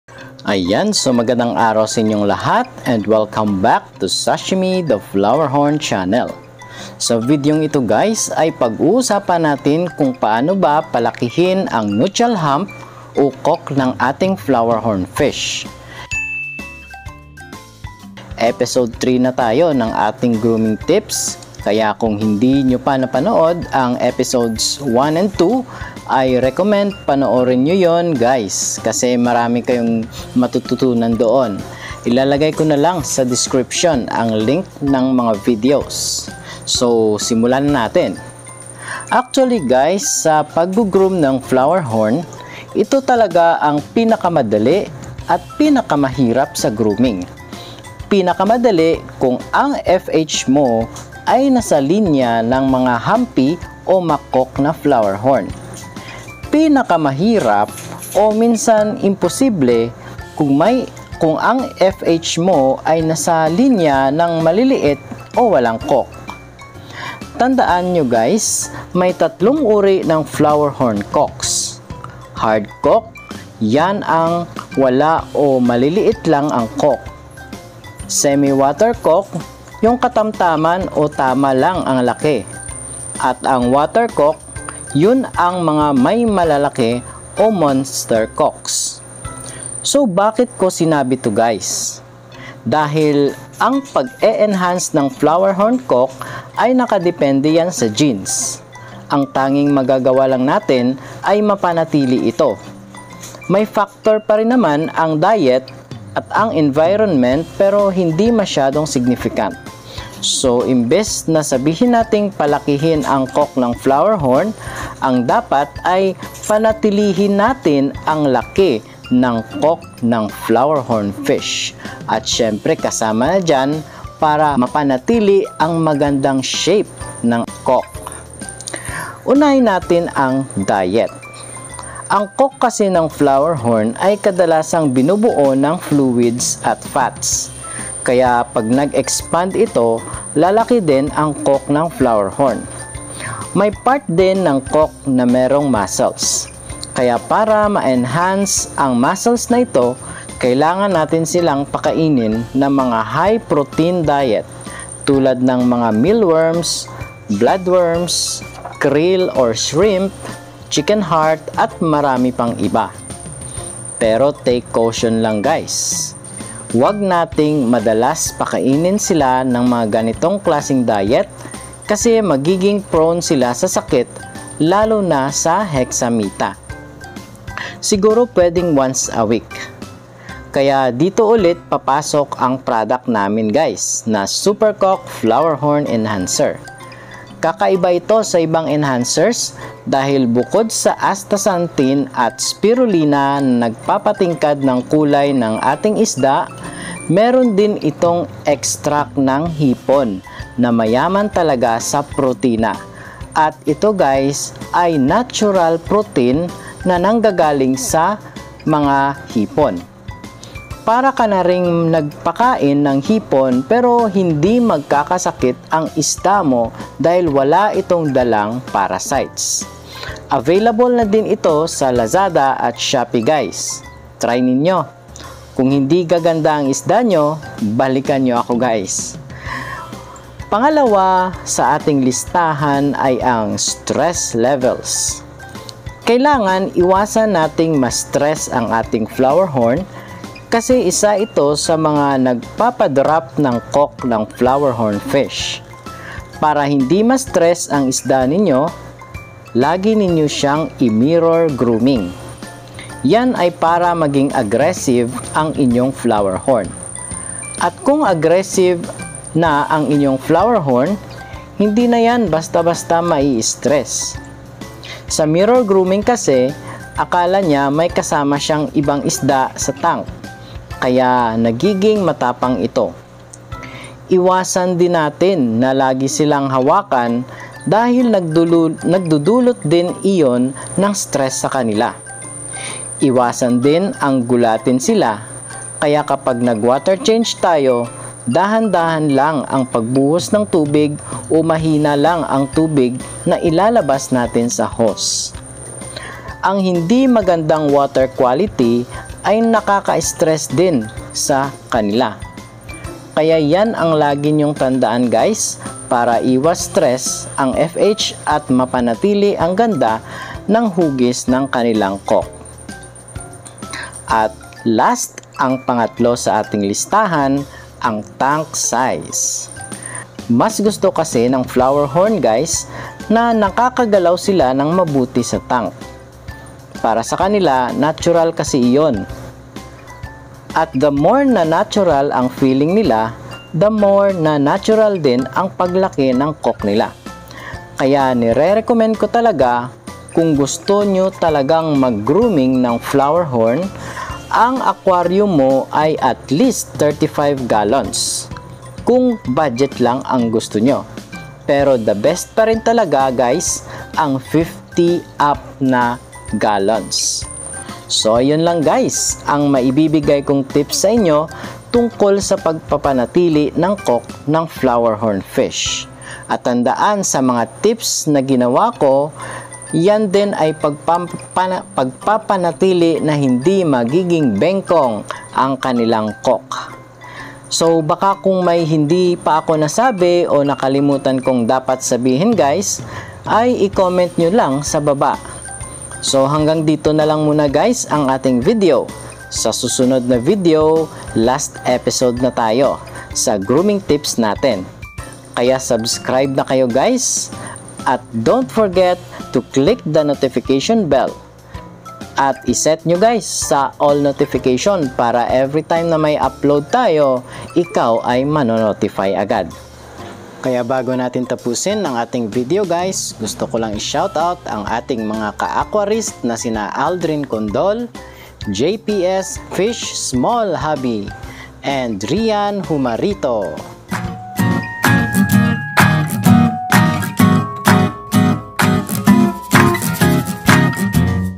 Paano Palakihin Ang Kok ng Flowerhorn Fish? Grooming Tips Ep. 3
Sa video na ito ay pag uusapan natin kung paano palakihin ang kok o nuchal hump ng ating flowerhorn fish.